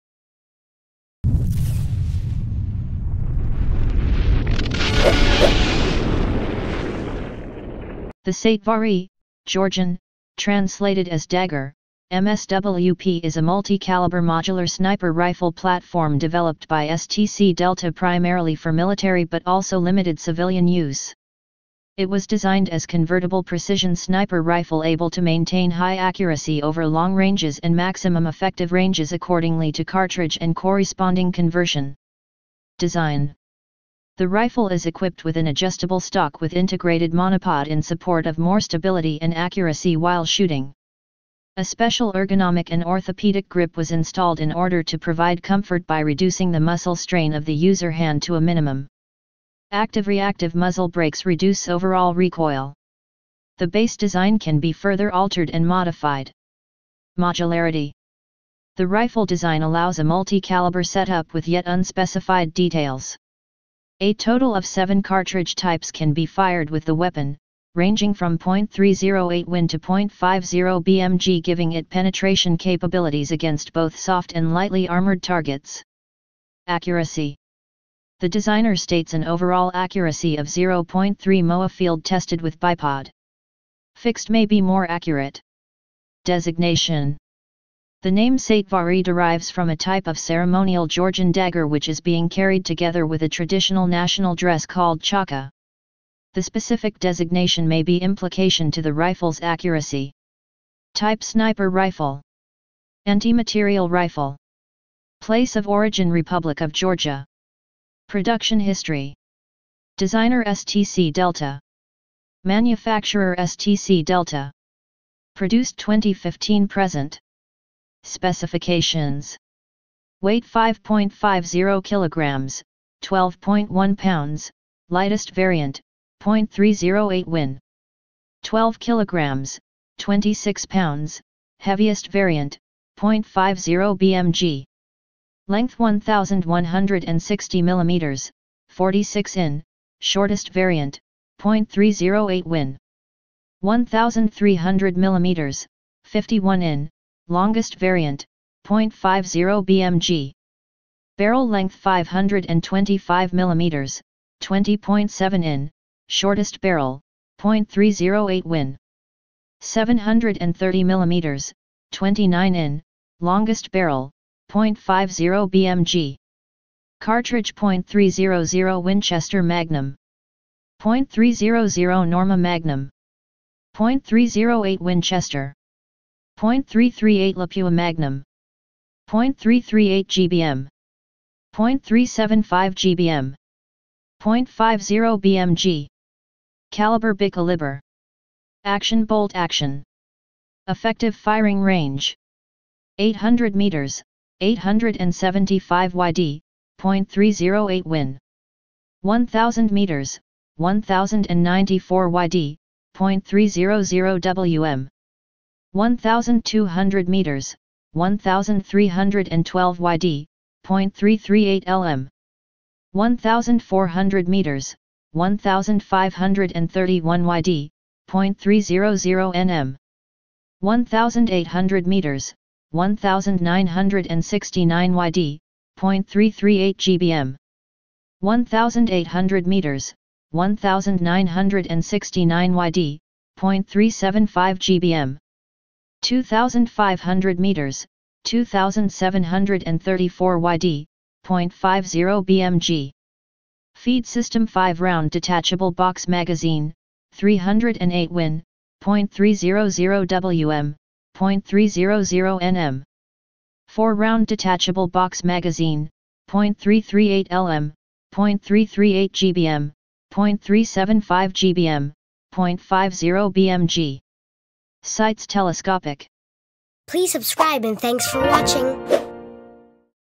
Satevari MSWP (Everything WEAPONRY & MORE)💬⚔️🏹📡🤺🌎😜✅
This video is about "Satevari MSWP". This video series is something special. We're fully delving into all things everything and all things about the everything you could imagine in WEAPONRY. From guns, war, ammo, strategy, bombs, destroyers, air craft carriers to the politics and manufacturers of weapons!!!